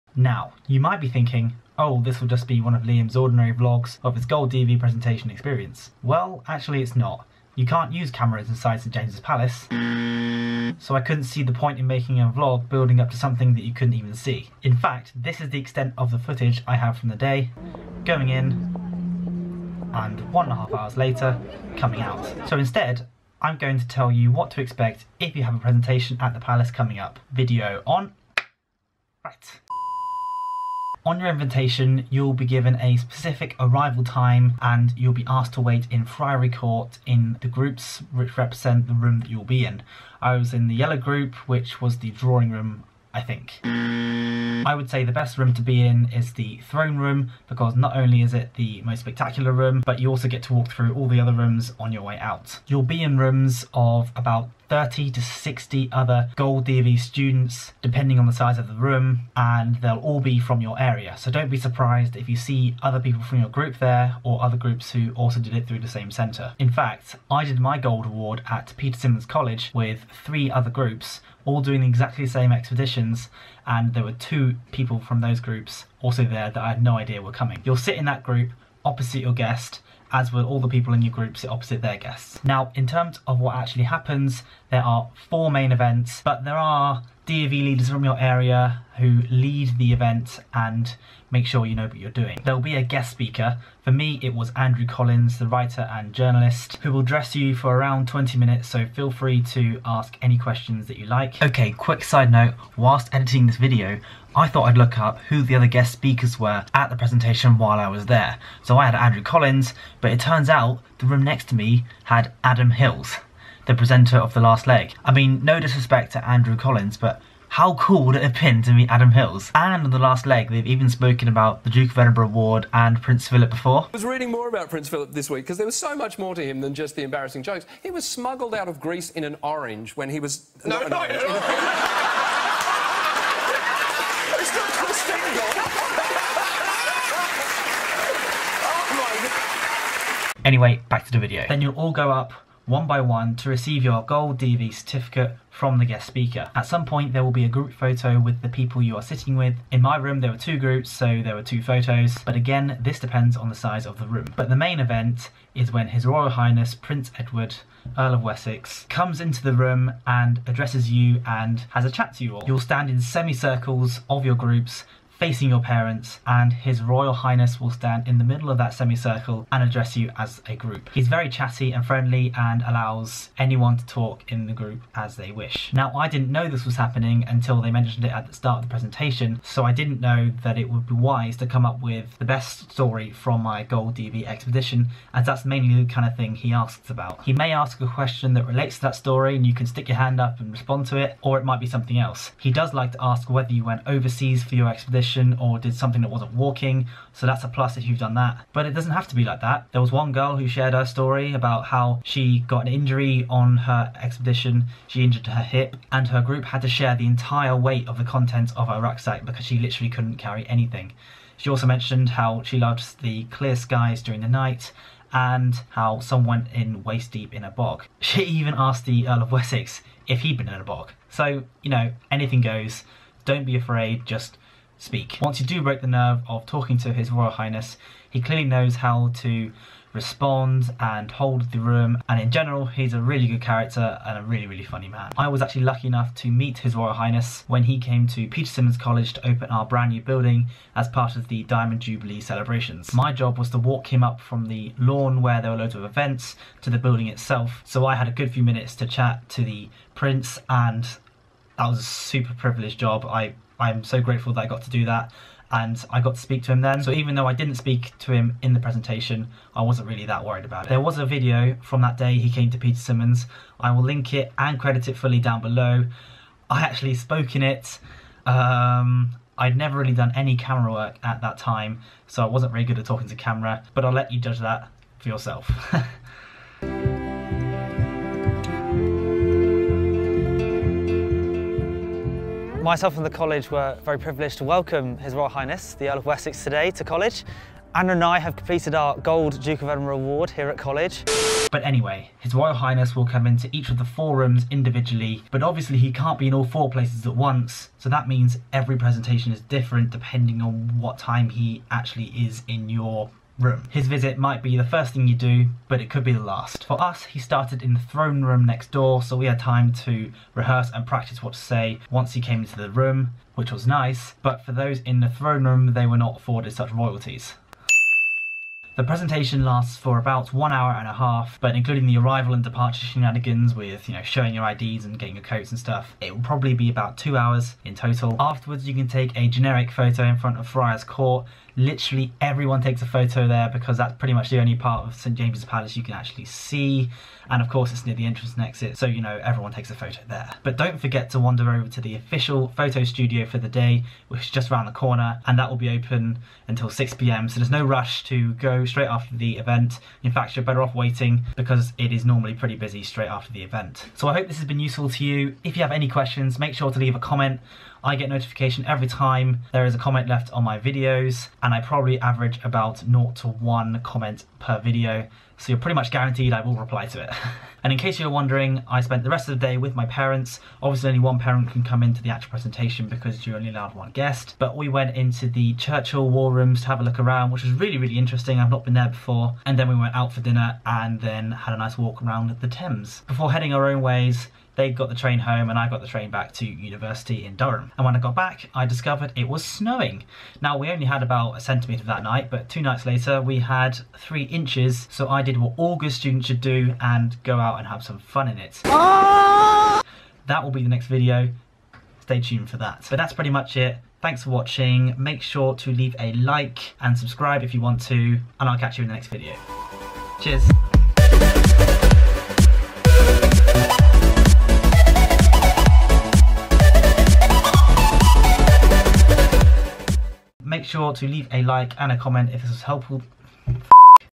What happens at St James's Palace? (Gold DofE Award Presentations 2018)
No cameras are allowed inside St James's Palace, so this is not your normal vlog. Instead, I explain everything that goes on at the Gold DofE Award Presentations 2018 based on my experience...

Sorry about the audio quality, I've realised my voice resonates between the walls surrounding my desk.

Filmed on 16 January 2018

Diamond Jubilee celebrations at Peter Symonds College (by Katy Garrett Photography, PSC Vimeo)

Support Me!

My tools!

*CHANNEL STATISTICS (at time of upload)*
Total Views: 37,150